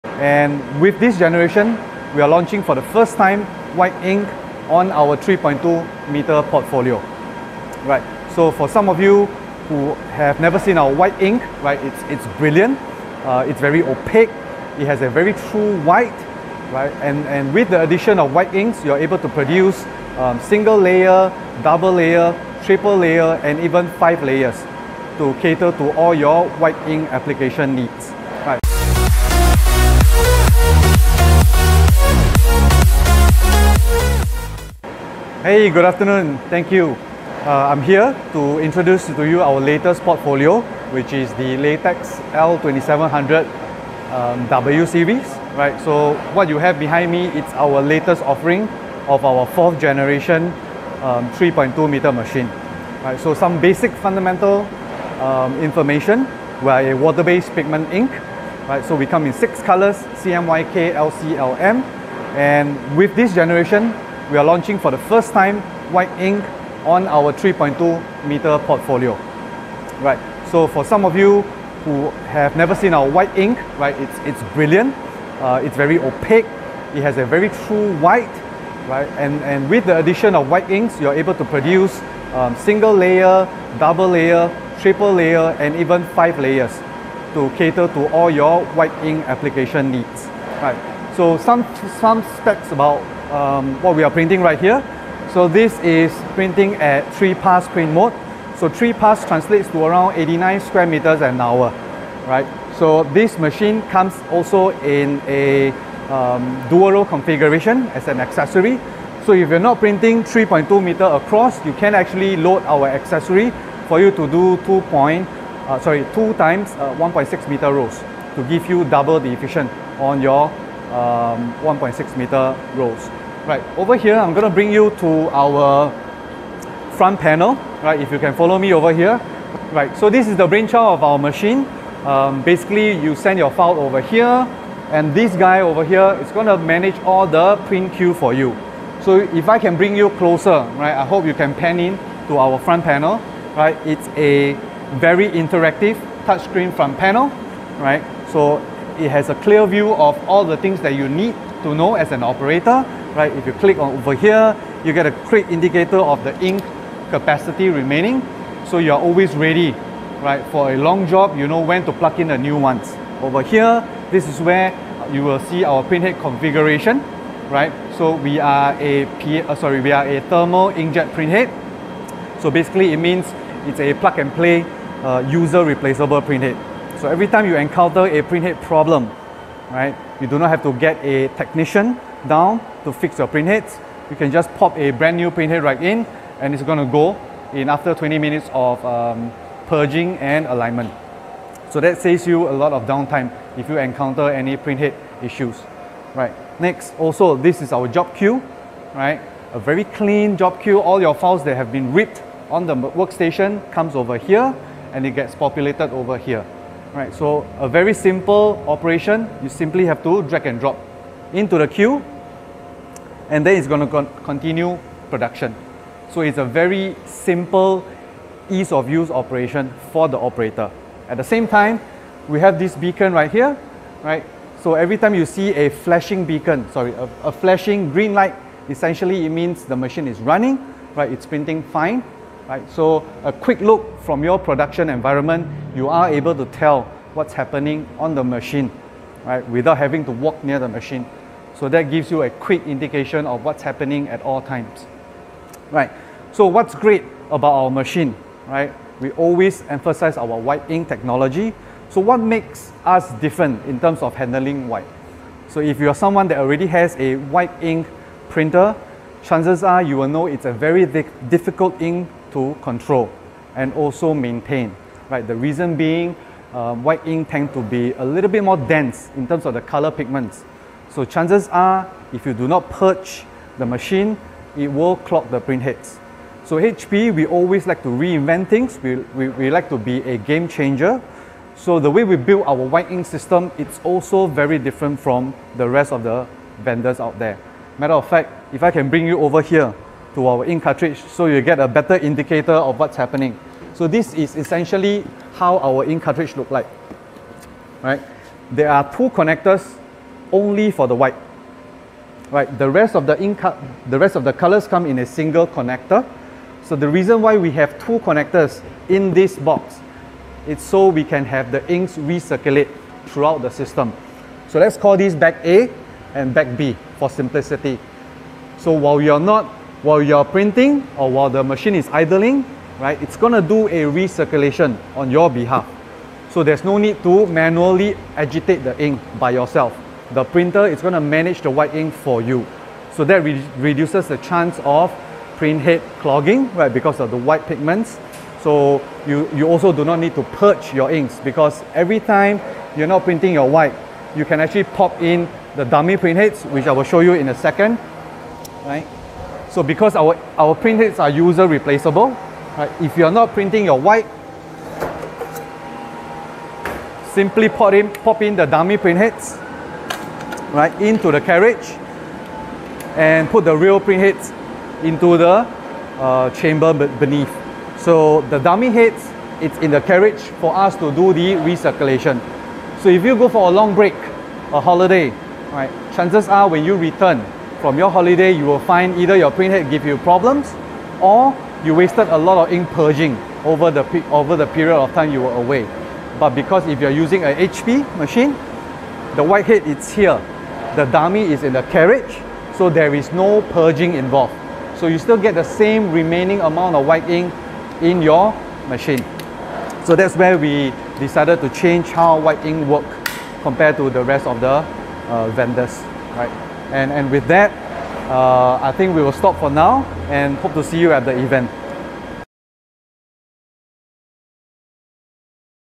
Technical Spesification HP Latex 2700 W
Interview Khusus Kelvin Peh, HP Large Format and Technical Consultant About HP Latex 2700 W